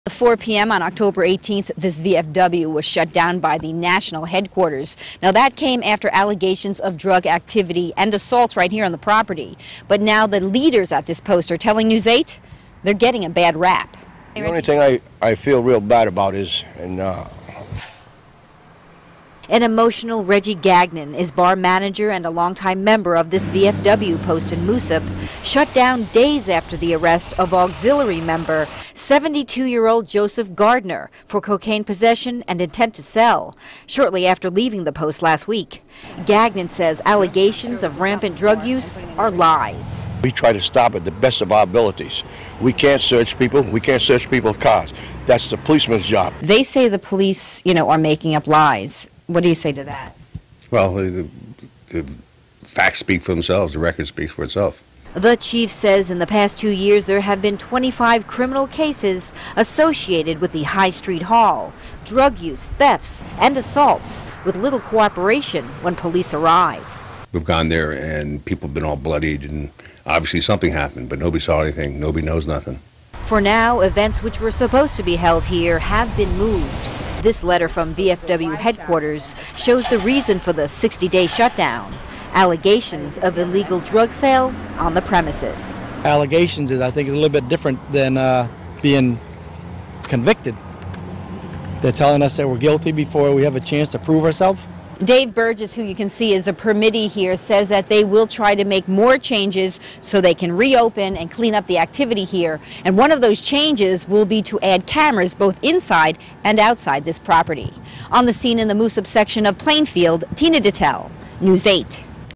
VFW shut down due to drug allegations
A local VFW in Moosup remains shut down, amid allegations of drug activity and assault.